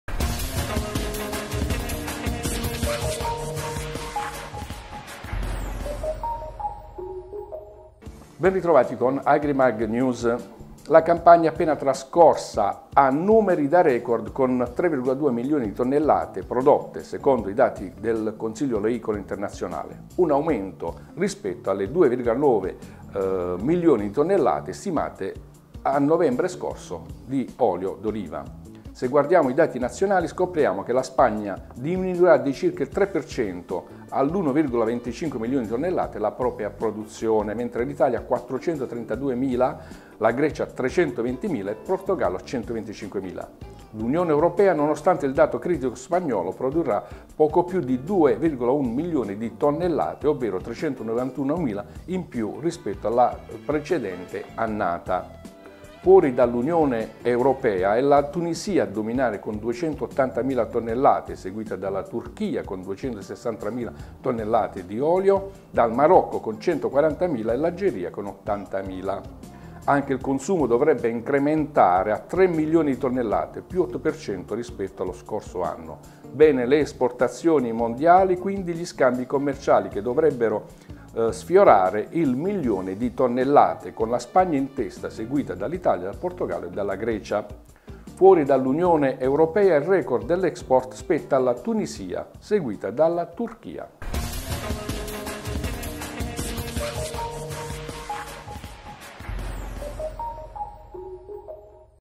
Agrimagnews | La produzione mondiale d'olio d'oliva a 3,2 milioni di tonnellate
La campagna appena trascorsa ha numeri da record con 3,2 milioni di tonnellate prodotte, secondo i dati Coi. Un aumento rispetto alle 2,9 milioni di tonnellate stimate a novembre scorso.

Se guardiamo i dati nazionali, scopriamo che la Spagna diminuirà di circa il 3% all'1,25 milioni di tonnellate la propria produzione, mentre l'Italia a 432 mila, la Grecia a 320 mila, il Portogallo a 125 mila. L'Unione europea, nonostante il dato critico Spagnolo, produrrà poco più di 2,1 milioni di tonnellate, ovvero 391 mila in più della scorsa campagna olearia.

Fuori dall'Unione europea è la Tunisia a dominare con 280 mila tonnellate, seguita dalla Turchia con 263 mila tonnellate, dal Marocco con 140 mila e l'Algeria con 80 mila.    Anche il consumo dovrebbe incrementare a 3 milioni di tonnellate, +8% rispetto allo scorso anno. Bene le esportazioni mondiali, quindi gli scambi commerciali, che dovrebbero sfiorare il milione di tonnellate, con la Spagna in testa, seguita da Italia, dal Portogallo e dalla Grecia.

Fuori dall'Unione europea il record dell'export spetta alla Tunisia, seguita dalla Turchia.